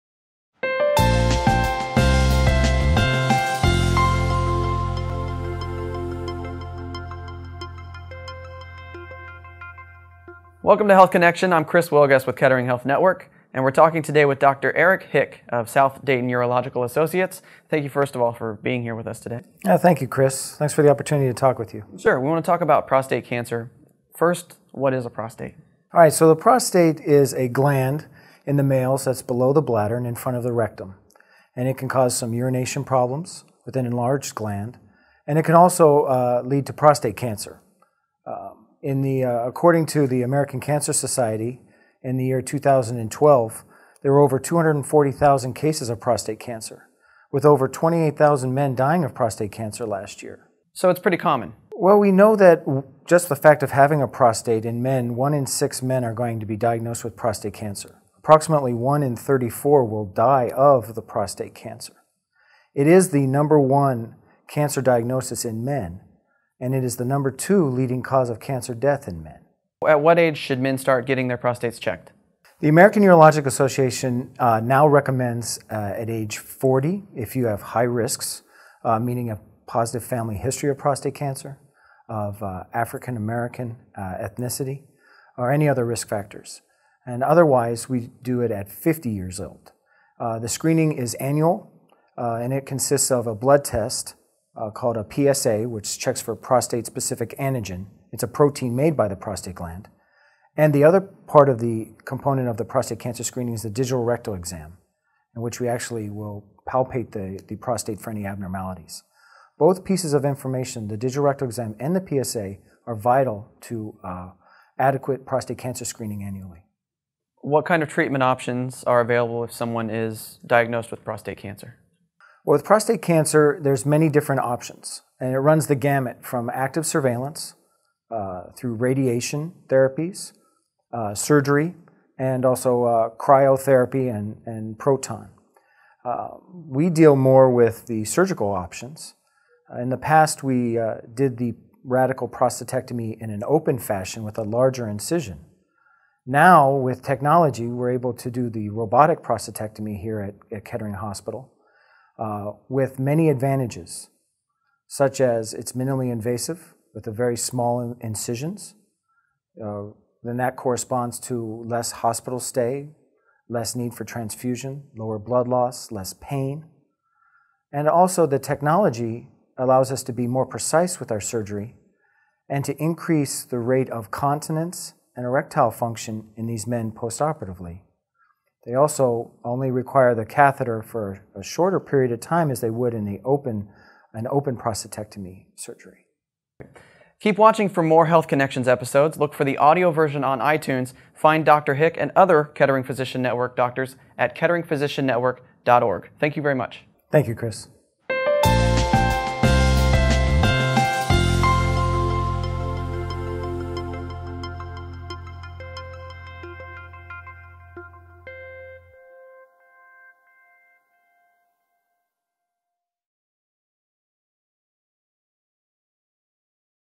Prostate Cancer - Screening and Treatment
Eric Hick, DO explains the prostate and how it can develop prostate cancer. With 1 in 6 men being diagnosed with prostate cancer, Dr. Hick discusses when to get checked and treatment options.